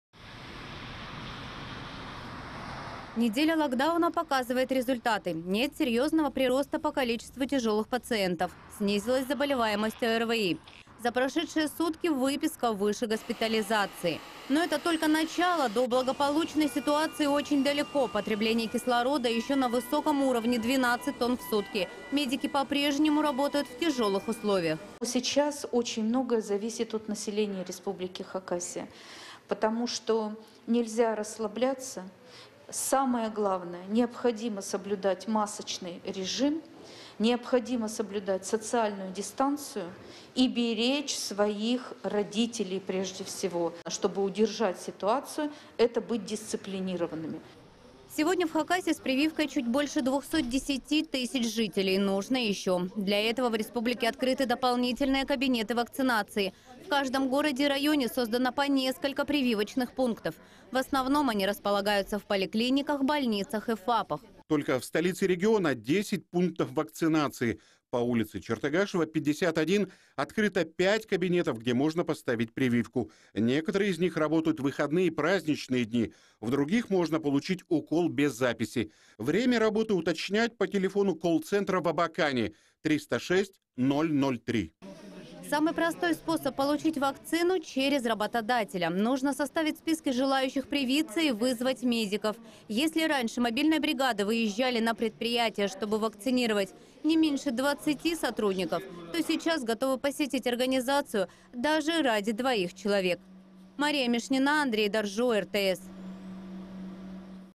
"Нельзя расслабляться": жителям Хакасии напоминают, что нужно соблюдать антиковидные меры
Неделя локдауна показывает результаты. Нет серьезного прироста по количеству тяжелых пациентов. Снизилась заболеваемость ОРВИ. За прошедшие сутки - выписка выше госпитализации. Но это только начало стабилизации, до благополучной ситуации - очень далеко. Потребление кислорода еще на высоком уровне - 12 тонн в сутки. Медики по-прежнему работают в тяжелых условиях.